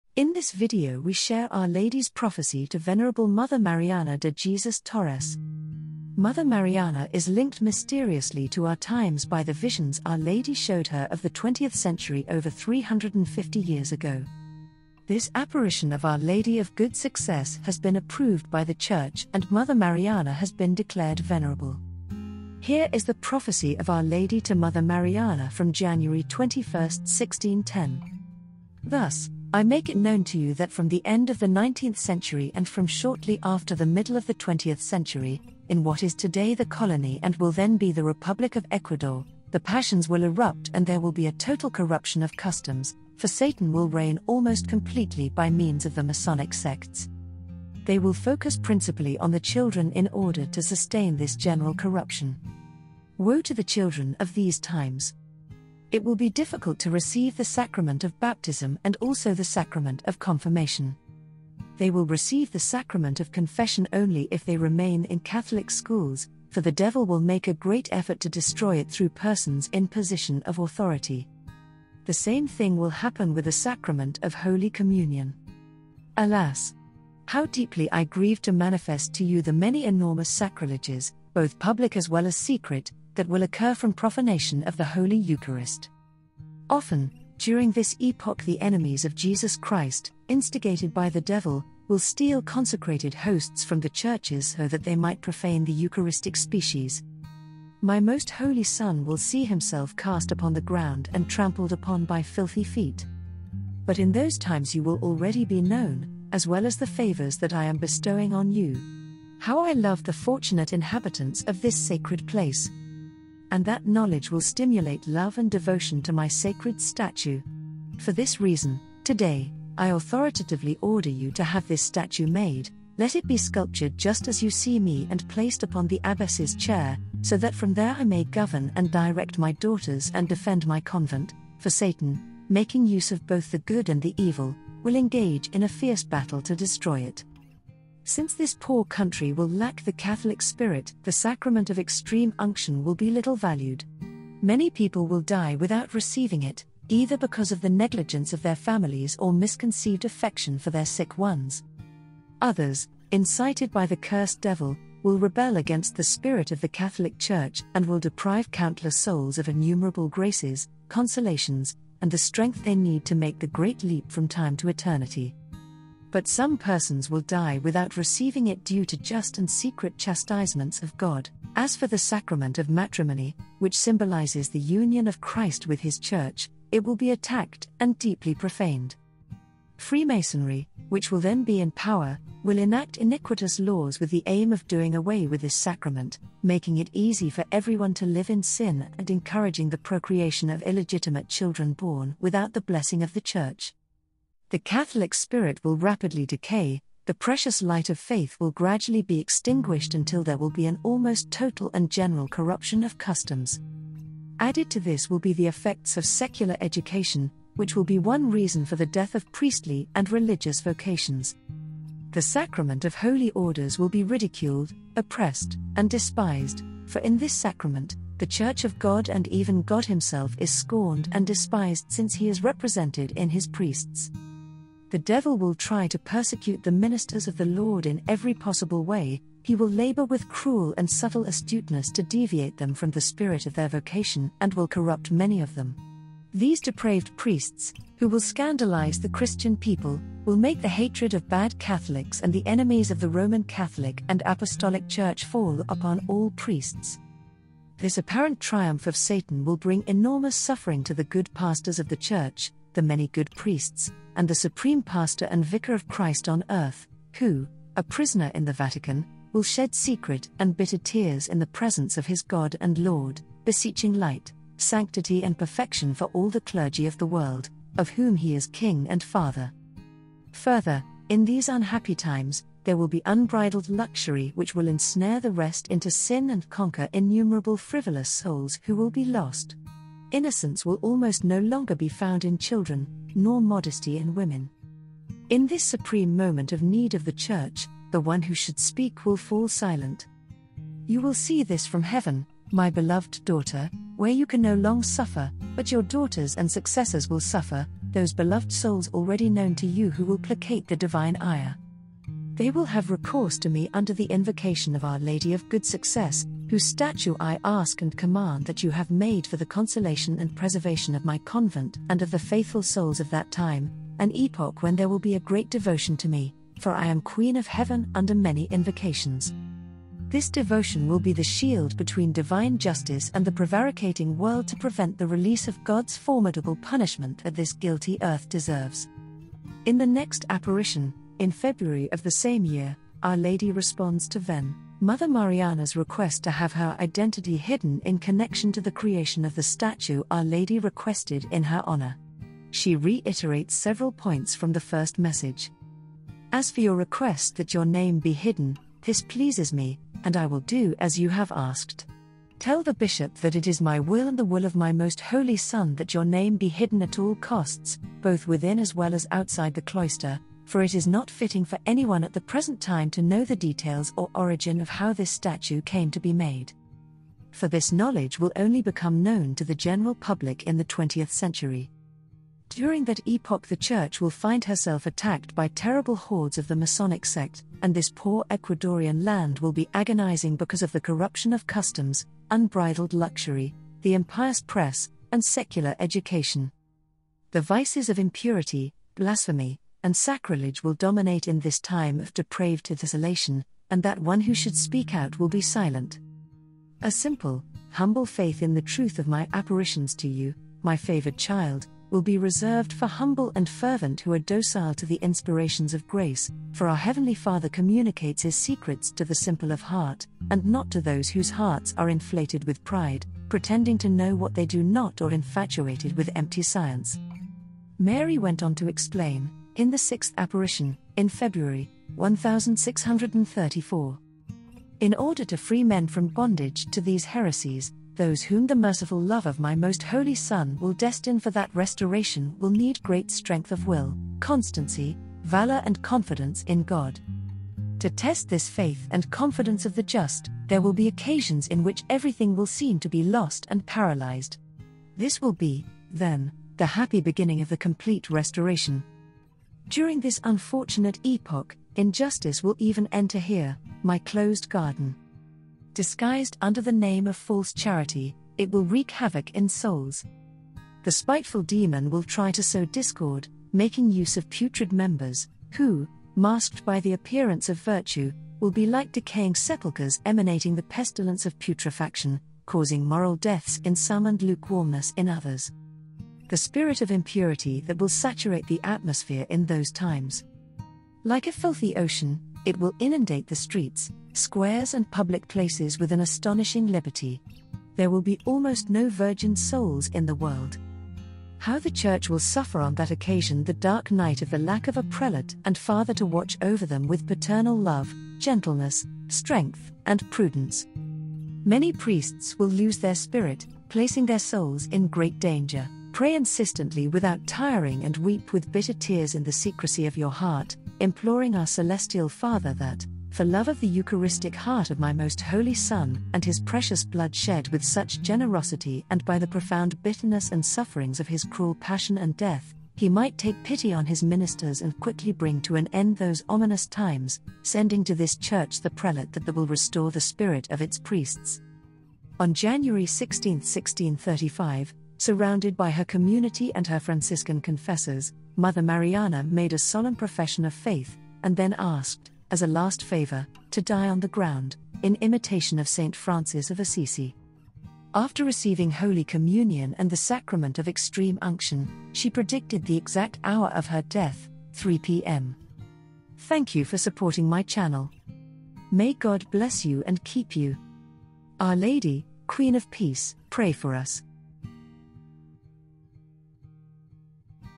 Our Lady's Prophecy to Venerable Mother Mariana de Jesus Torres
In this video we share Our Lady's Prophecy to Venerable Mother Mariana de Jesus Torres

Mother Mariana is linked mysteriously to our times by the visions Our Lady showed her of the 20th Century over 350 years ago.

This apparition of Our Lady of Good Success has been approved by the Church and Mother Mariana has been declared Venerable.

Here is the prophecy of Our Lady to Mother Mariana from January 21, 1610:

“Thus,  I make it known to you that from the end of the 19th century and from shortly after the middle of the 20th century,  in what is today the Colony and will then be the Republic of Ecuador,  the passions will erupt and there will be a total corruption of customs,  for Satan will reign almost completely by means of the Masonic Sects.

They will focus principally on the children in order to sustain this general corruption.

Woe to the children of these times!

It will be difficult to receive the Sacrament of Baptism and also the Sacrament of Confirmation.

They will receive the Sacrament of Confession only if they remain in Catholic schools,  for the Devil will make a great effort to destroy it through persons in position of authority.

The same thing will happen with the Sacrament of Holy Communion.

Alas!  How deeply I grieve to manifest to you the many enormous sacrileges – both public as well as secret – that will occur from profanation of the Holy Eucharist.

Often,  during this epoch the enemies of Jesus Christ,  instigated by the Devil,  will steal consecrated Hosts from the churches so that they might profane the Eucharistic Species.

My Most Holy Son will see Himself cast upon the ground and trampled upon by filthy feet.

But in those times you will already be known,  as well as the favors that I am bestowing on you.  How I love the fortunate inhabitants of this sacred place!

And that knowledge will stimulate love and devotion to my Sacred Statue.  For this reason,  today,  I authoritatively order you to have this Statue made: let it be sculptured just as you see Me and placed upon the Abbess’s chair,  so that from there I may govern and direct my daughters and defend my Convent; for Satan,  making use of both the good and the evil,  will engage in a fierce battle to destroy it.

Since this poor Country will lack the Catholic spirit,  the Sacrament of Extreme Unction will be little valued.  Many people will die without receiving it – either because of the negligence of their families or misconceived affection for their sick ones.

Others,  incited by the cursed Devil,  will rebel against the spirit of the Catholic Church and will deprive countless souls of innumerable graces,  consolations,  and the strength they need to make the great leap from time to eternity.

But some persons will die without receiving it due to just and secret chastisements of God.

As for the Sacrament of Matrimony,  which symbolises the union of Christ with His Church,  it will be attacked and deeply profaned.